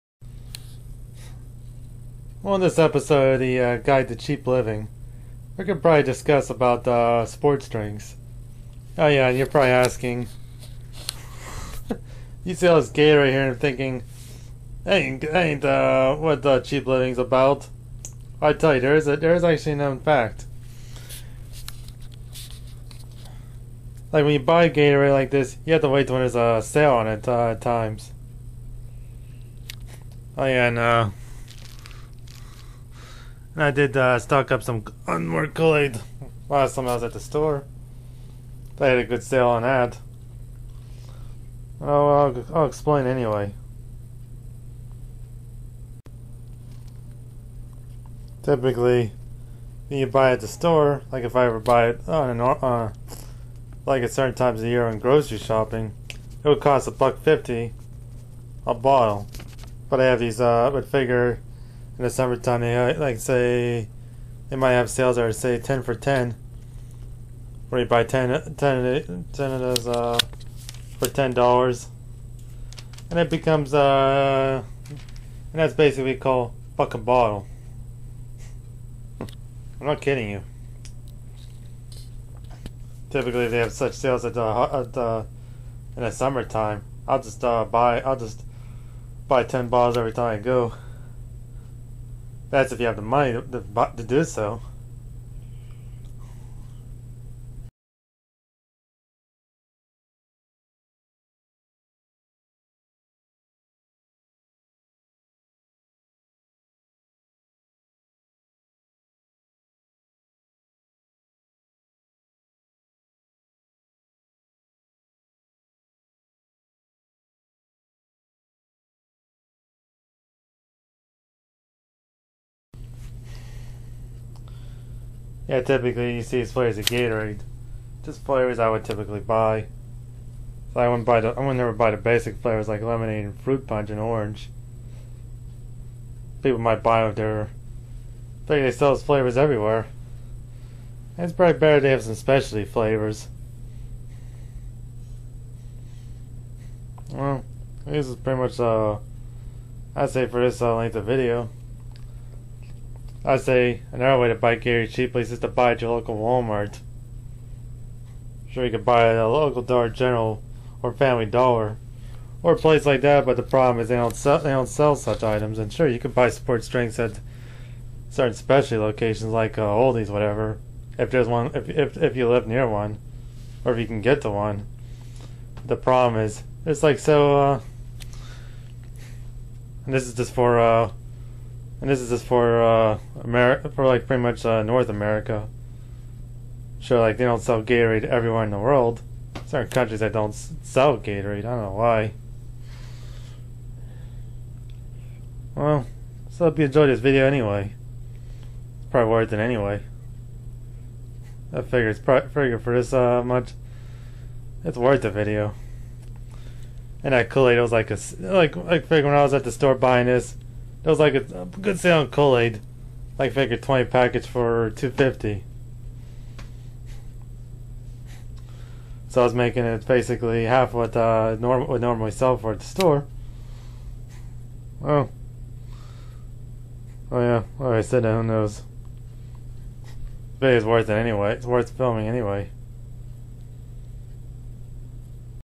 Guide To Cheap Living Episode 3
Just Discussing On How To Get Sports Drinks Cheaply.